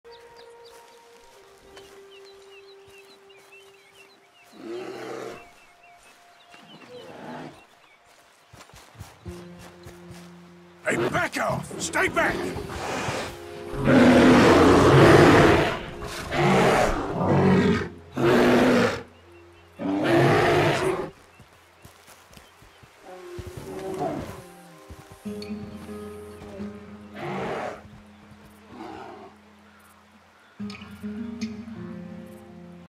Bear face off
Arthur face off a bear and stare him down. :)